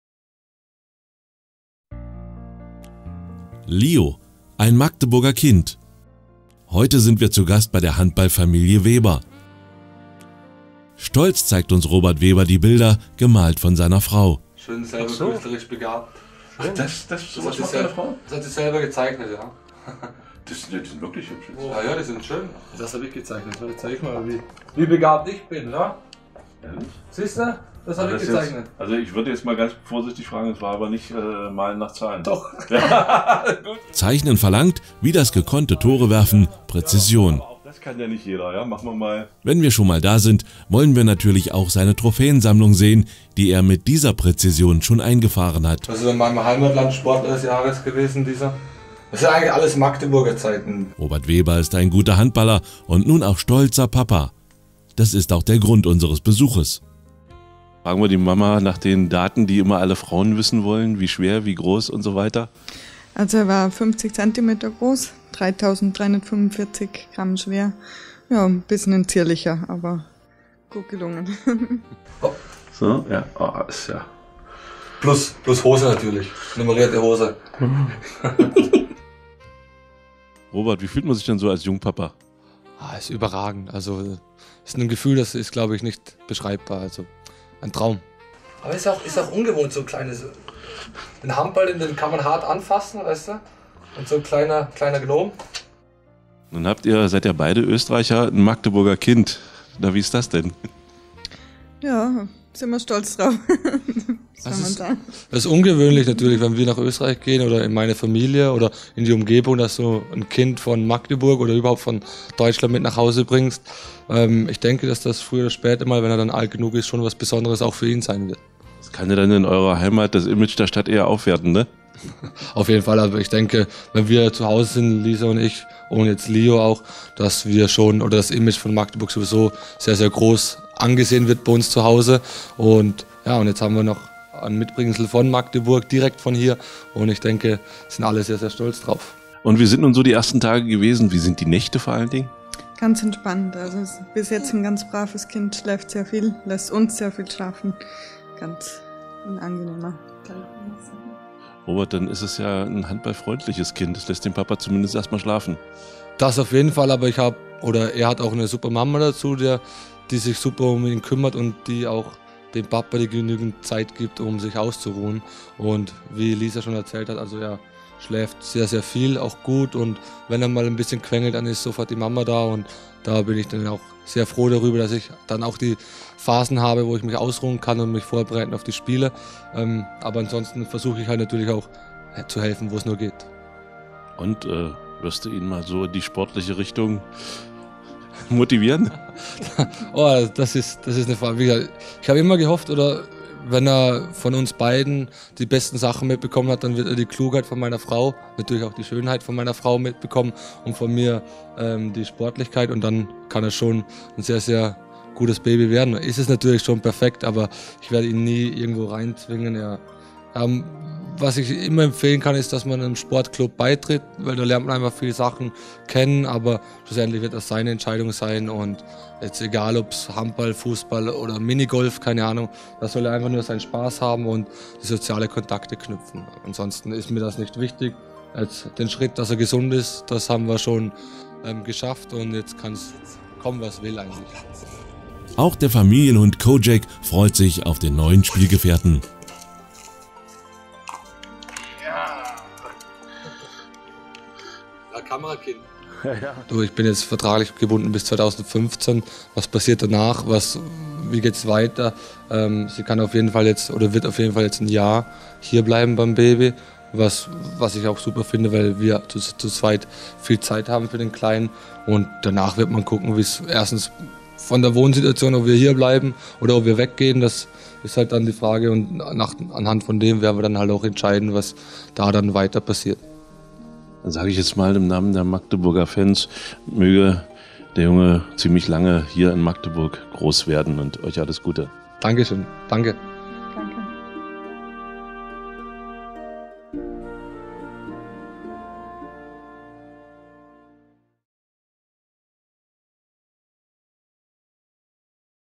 Wir besuchen Lisa und Rober Weber mit Söhnchen Lio
Mal wieder was für´s Herz. Unser SCM-Handball-Flügelflitzer Robert Weber ist Papa eines Sohnes geworden. Am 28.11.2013 haben wir ihn, seine Frau Lisa und Söhnchen Lio besucht. Hier der Bericht von SCM-TV!